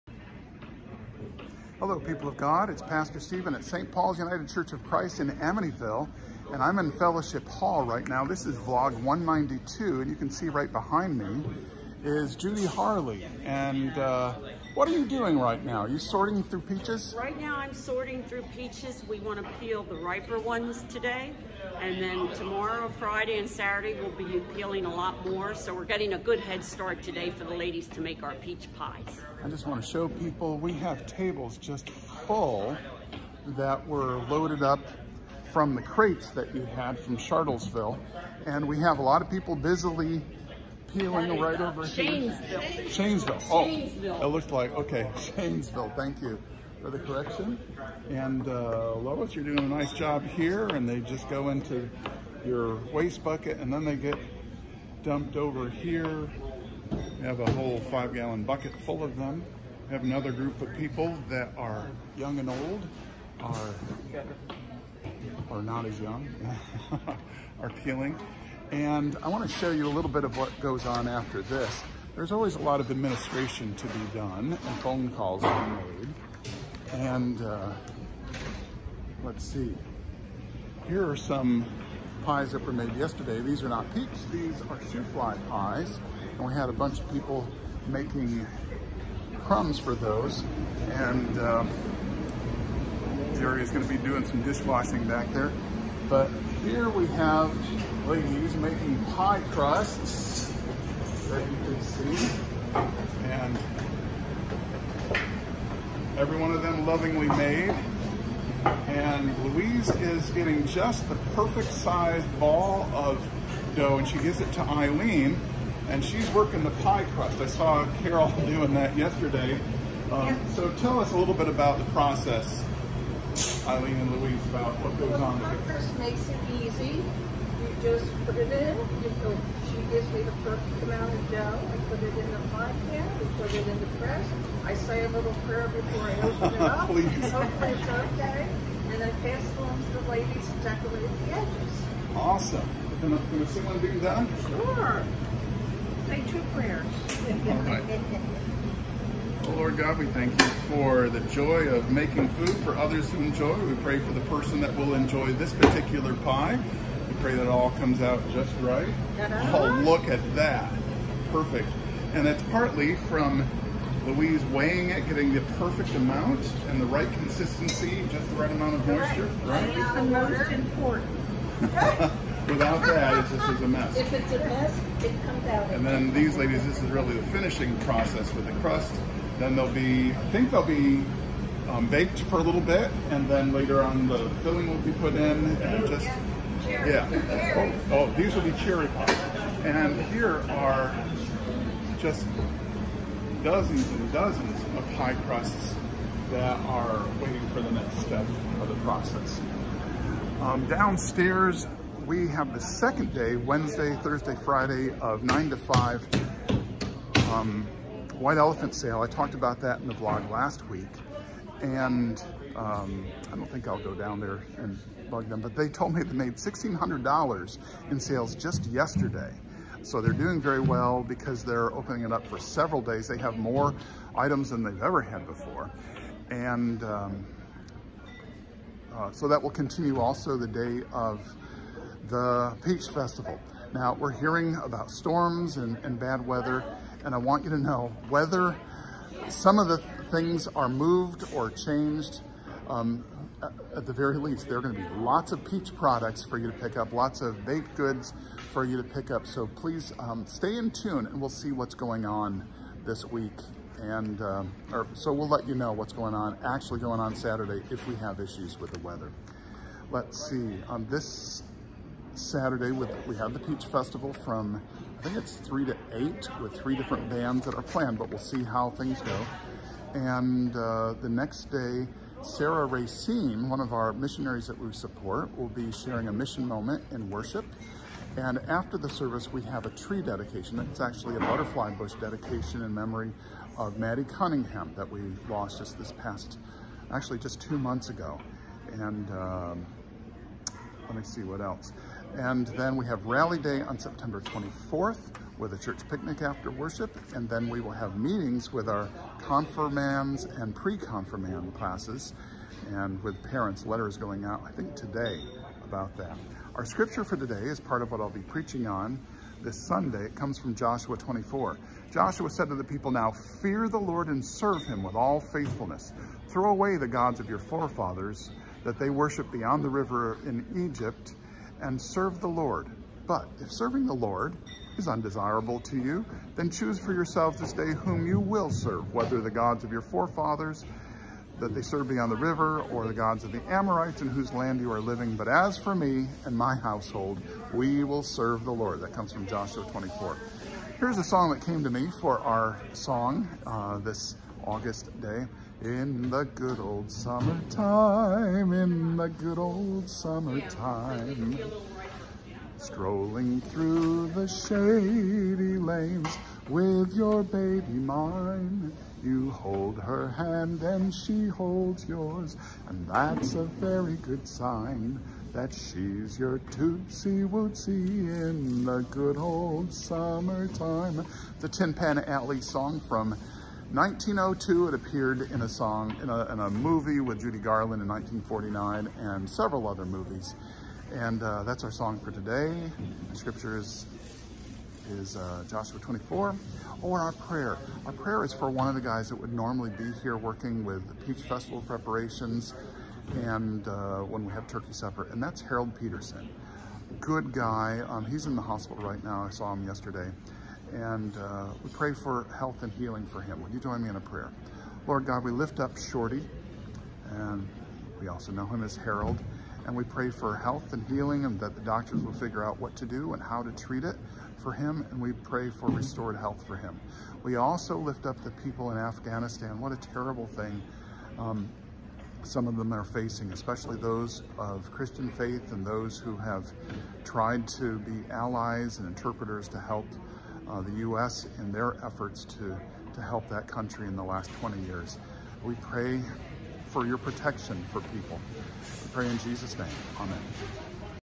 Vlog #192 Peach Preparations
Pastor Steven's weekly video log talking with the congregation about what's up at St. Paul's United Church of Christ in Amityville.